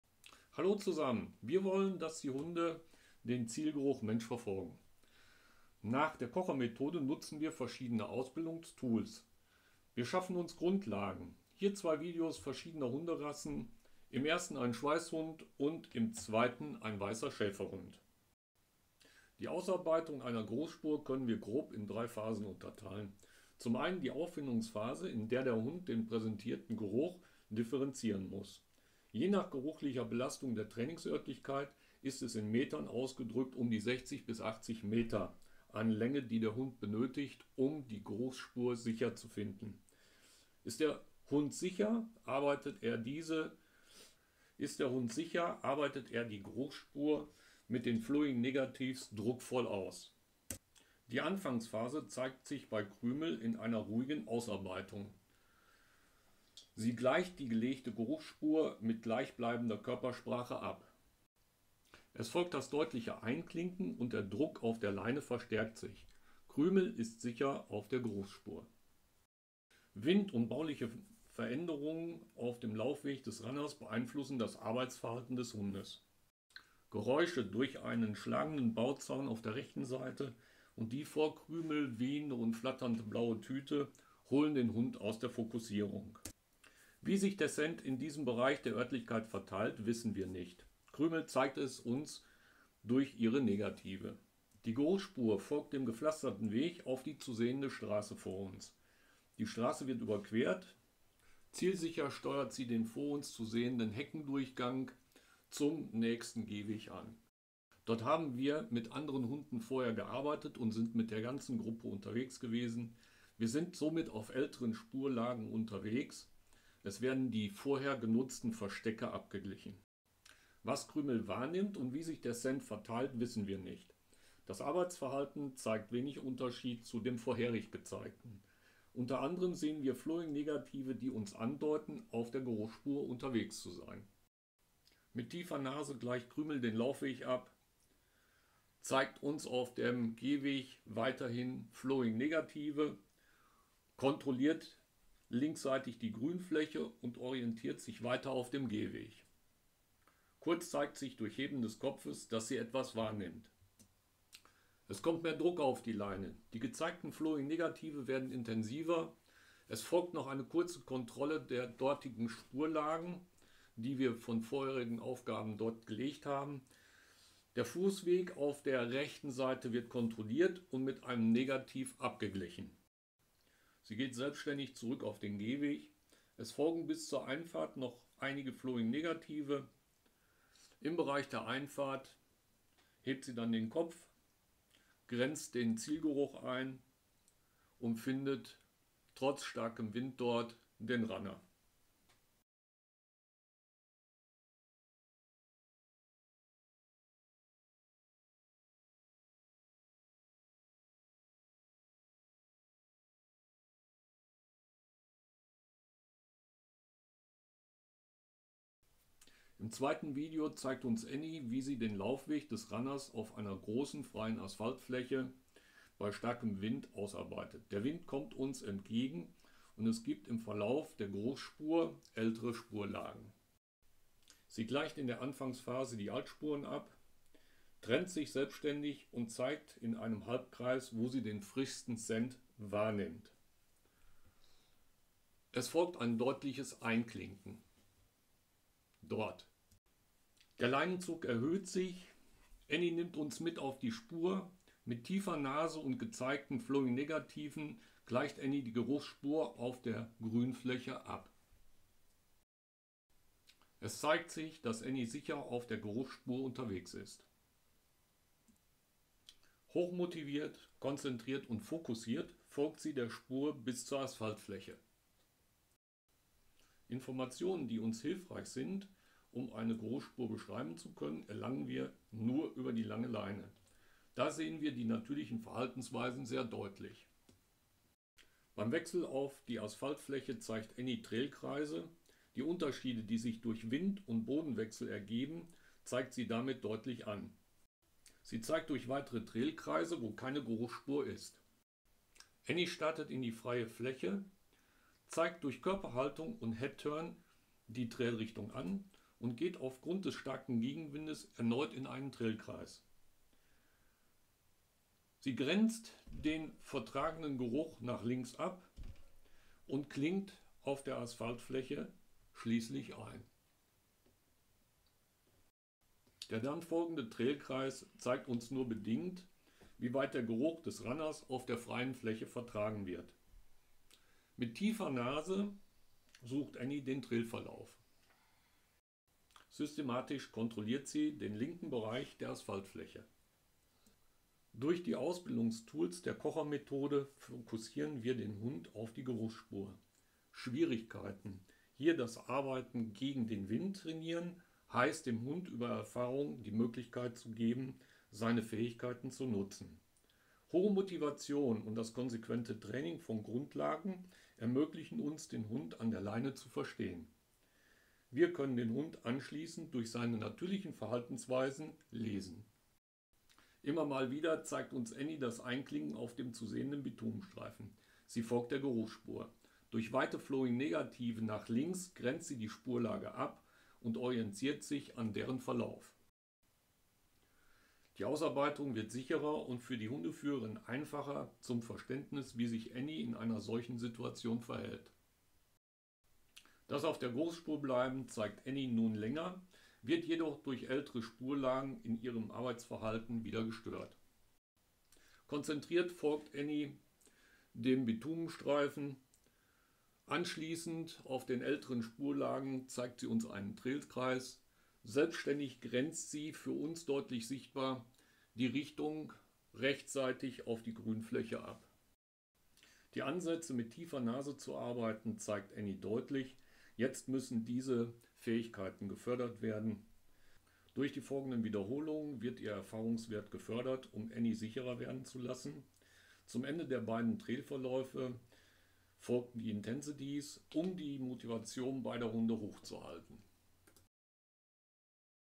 Mantrailing - Mit der Nase dicht am Geruch - Die Kocher Methode
Das Lesen des Hundes ist nach der Kocher Methode recht einfach und lässt sich auf jede Hunderasse anwenden. Mit einer klaren Grundlagenarbeit und verschiedenen Motivationstools(Intensity, Delayed Start Intensity, Scent Article Start Intensity) wird der Hund konzentriert und fokussiert auf die Geruchsspur gebracht. Die gezeigten natürlichen Verhaltensweisen, wenn der Hund eine Spur verliert, sind recht eindeutig und einfach. Mit einem klaren Grundlagentraining erreichen wir, dass der Hund uns vermittelt, wie eine Geruchsspur verläuft.

"Fortschritt drückt sich nicht im Alter oder der Länge eines Trails aus, sondern in der Stabilität der Grundlage." Kevin Kocher